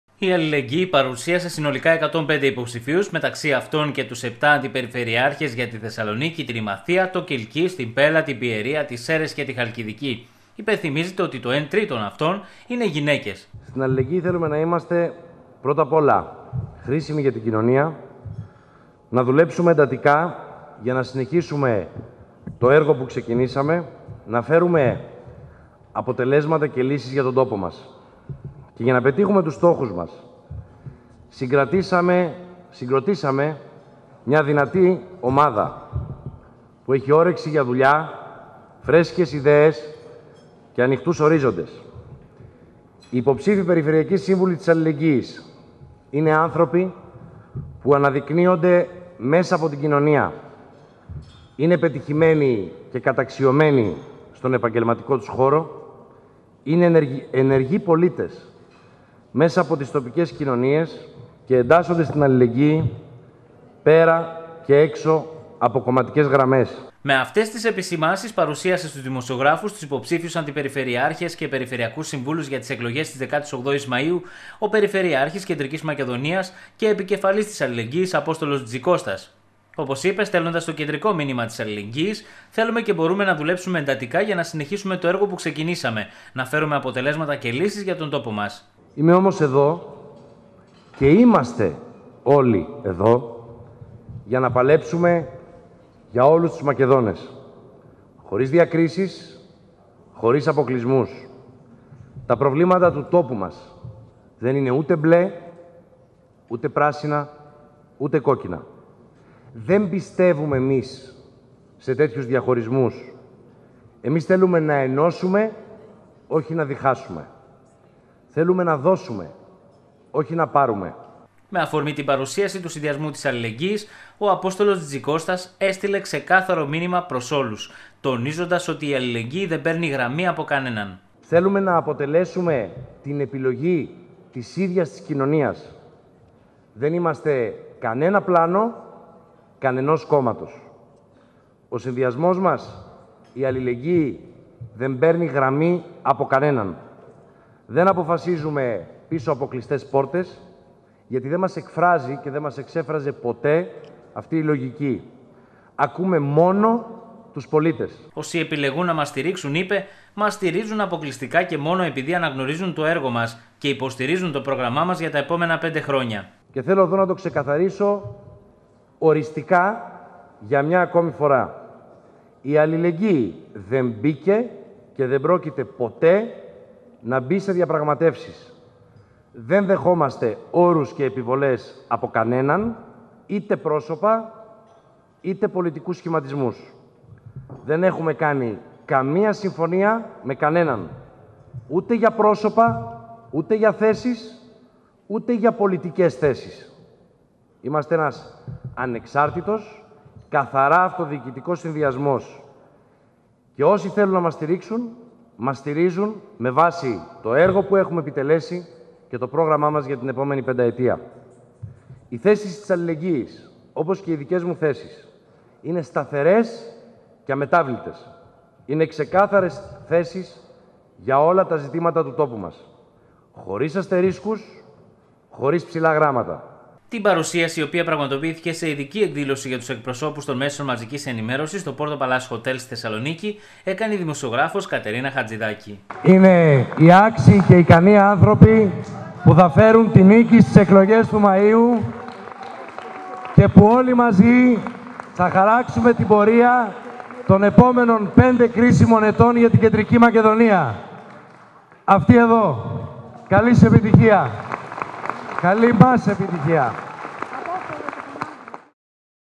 ΑΠΟΣΤΟΛΟΣ ΤΖΙΤΖΙΚΩΣΤΑΣ
Οι υποψήφιοι Αντιπεριφερειάρχες και Περιφερειακοί Σύμβουλοι της "Αλληλεγγύης" είναι άνθρωποι που αναδεικνύονται μέσα από την κοινωνία.  Είναι πετυχημένοι και καταξιωμένοι, στον επαγγελματικό τους χώρο. Είναι ενεργοί πολίτες πέρα και έξω από κομματικές γραμμές. Είναι άνθρωποι που γνωρίζουν καλά την αυτοδιοίκηση. Επαγγελματίες, επιστήμονες. Αυτά τόνισε ο Απόστολος Τζιτζικώστας, παρουσιάζοντας τους υποψηφίους για την περιφέρεια σε εκδήλωση που πραγματοποίηθηκε στη Θεσσαλονίκη.